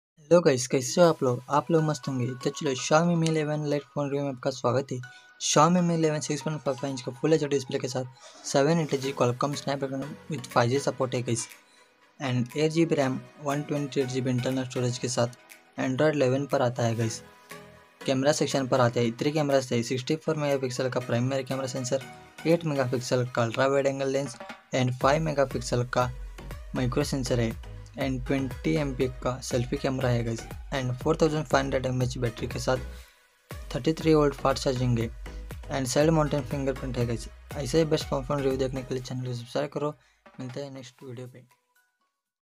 Xioami Mi 11 Lite / Xioami Mi 11 lite Review/ Xioame Mi 11 Camera test # phone review
Hi Dosto Xioami Mi 11 Lite phone review kia hai Jo DUBAI mein Available Hogaya hai fracture ye hai

Screen Size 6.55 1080P AMOLED 90 HZ
Snapdragon 732G
4250MAh Battery
UFS 2.2
Rear 64MP 8MP 5MP
Selfie 20MP

Milte hai next video pe